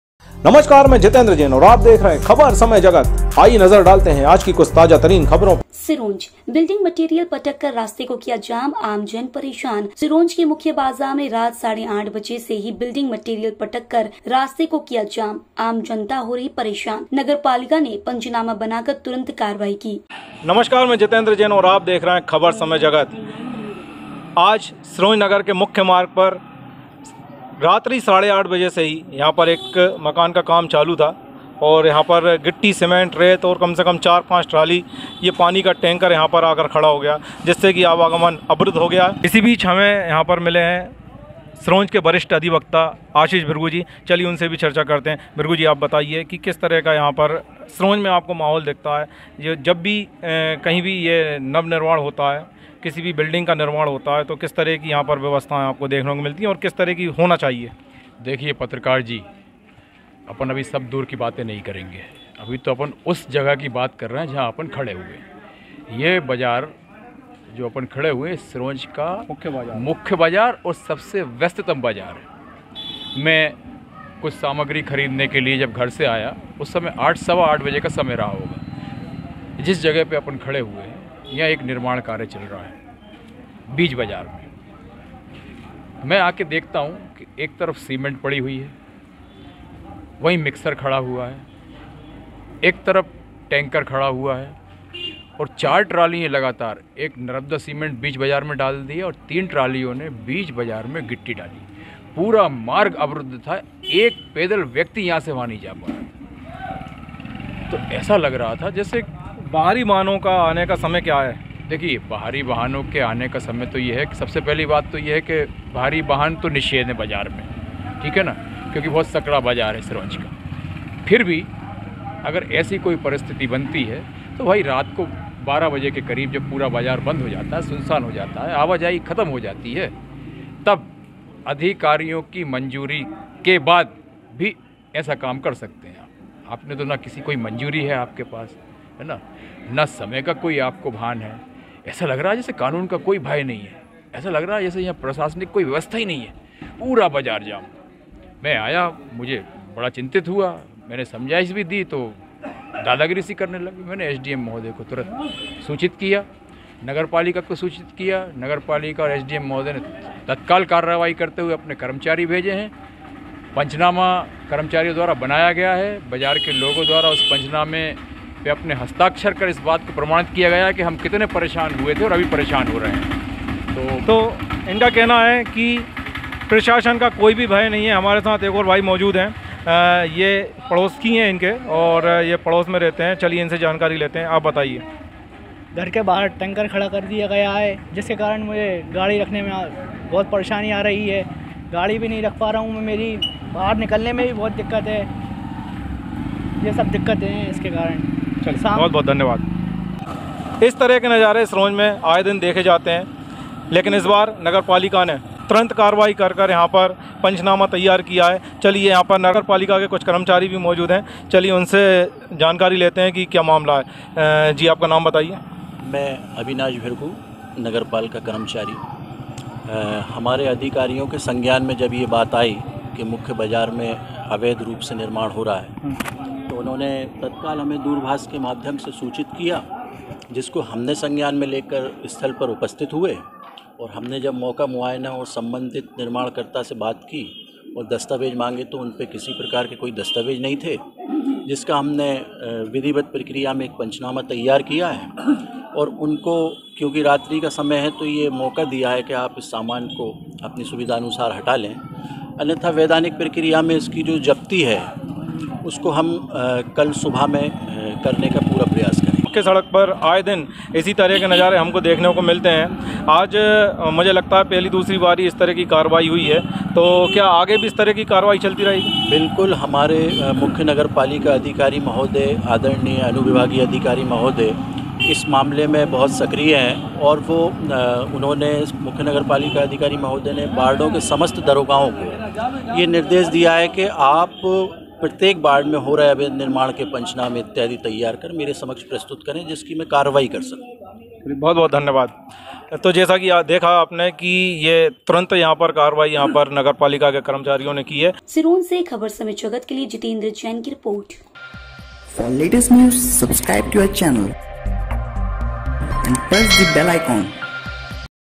सिरोंज:- बिल्डिंग मटेरियल पटक कर रास्ते को किया जाम, आमजन परेशान !!
जितेंद्र जैन की रिपोर्ट